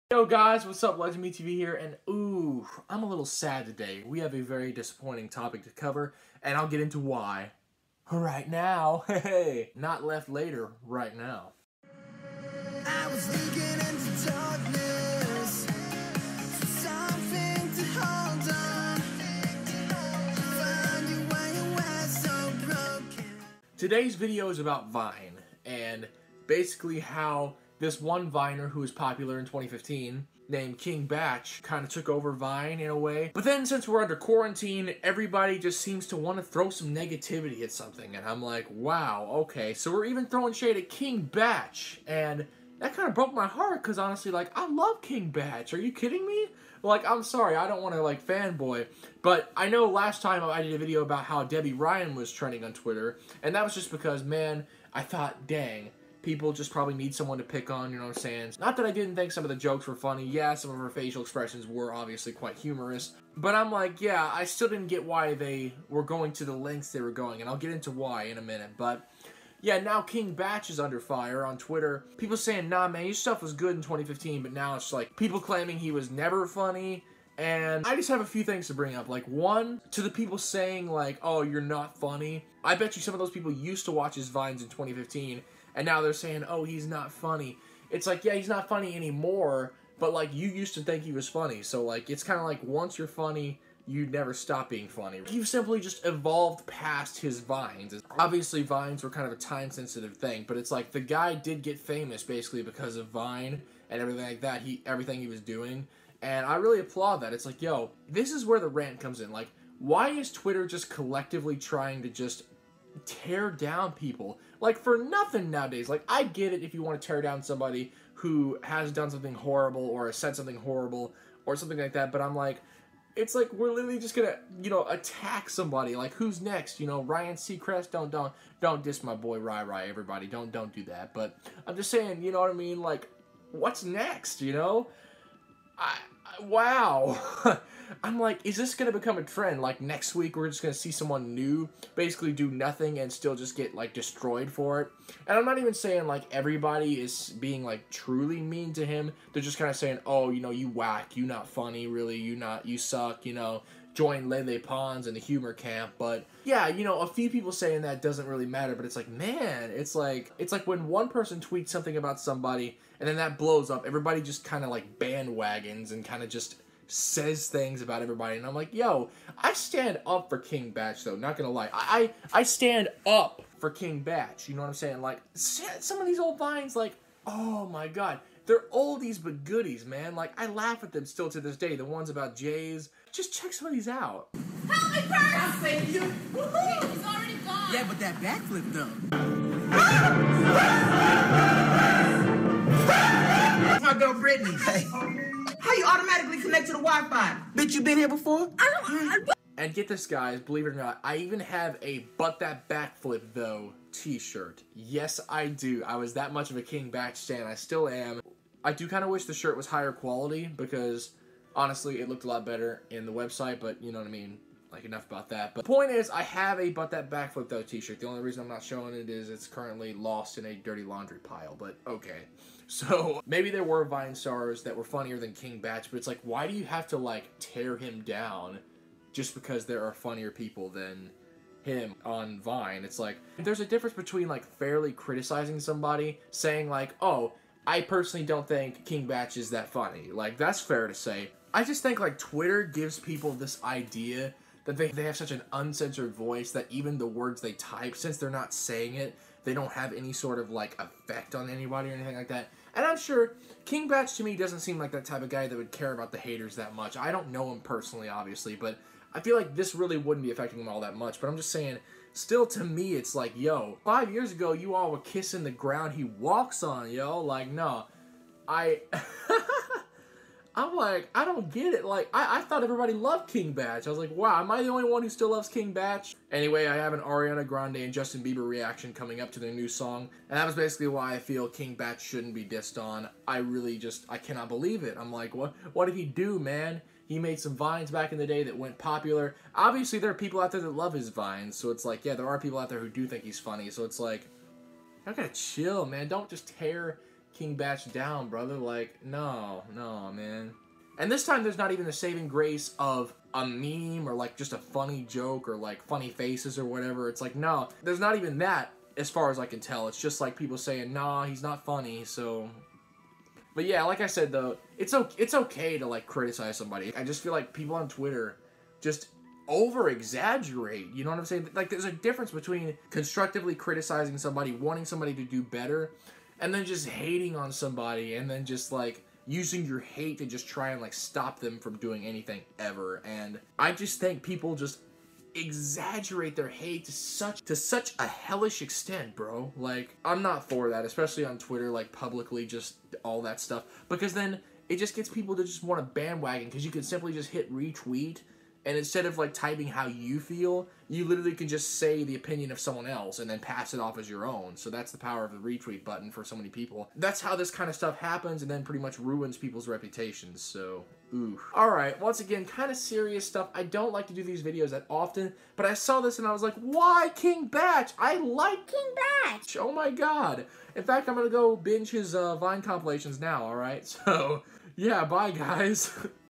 King Bach Trending: Twitter Declares He Was Never Funny
So we all know that Vine came and left us. With TikTok dominating as a short video platform, we are left to forget our Vine memories. Until today, on Twitter, King Bach was in the trending section and it turns out people, out of nowhere, were saying he was never funny, either that or people saying he isn't funny anymore.

Today I discuss why I feel it is okay to criticize someone, but why I feel all of this criticism turns to hate on twitter and why it is over exaggerated and unfair.